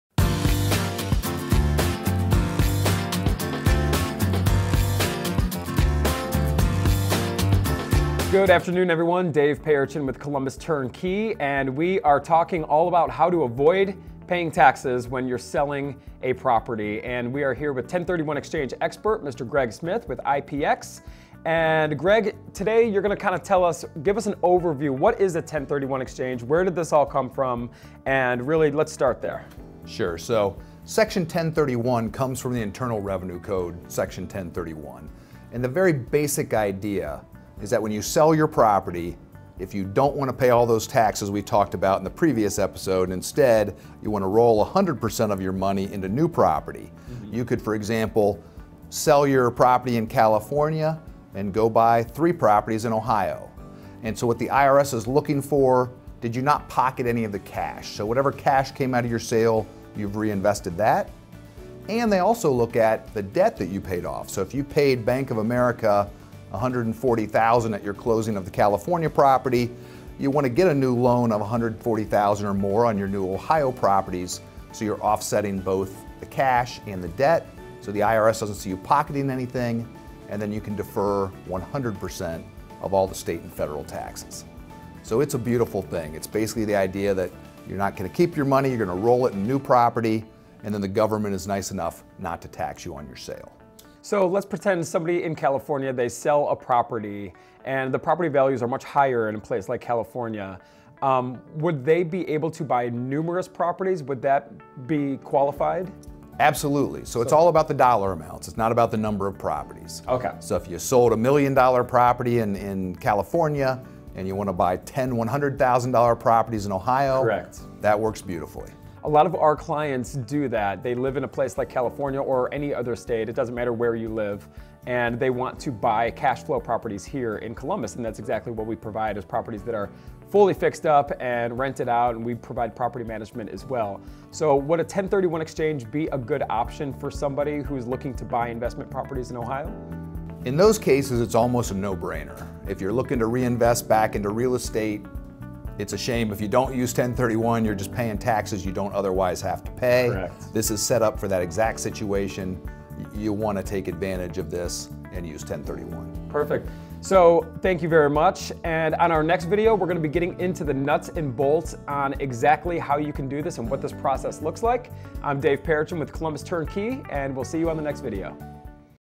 How To Avoid Paying Taxes using 1031 Exchange | Video (2/5)
| Considering Selling an investment property but don't want to get stuck with all the taxes.  Try using a 1031 Exchange.  Clients use the 1031 Exchange all the time when selling/acquiring investment properties.  If you're looking for investment properties, find us